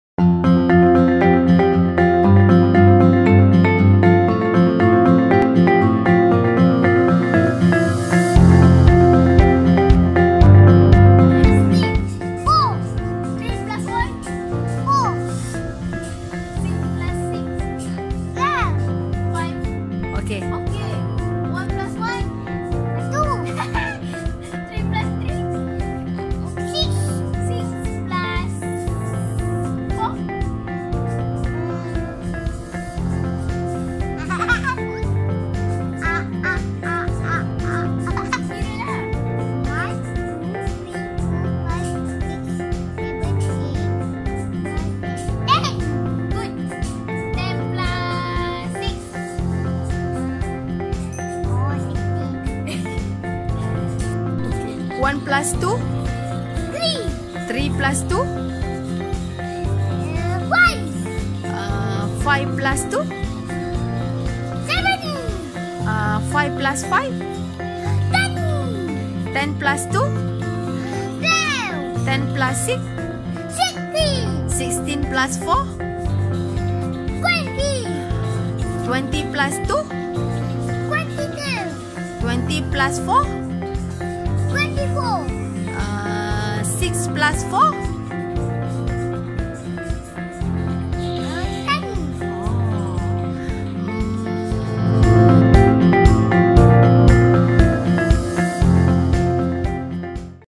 Adding Numbers With Ici
Adding Numbers With Ici. Paling suka ici layan siri animasi numbers block. Lepas tu dia akan minta tanya dia pula. Memang seronok.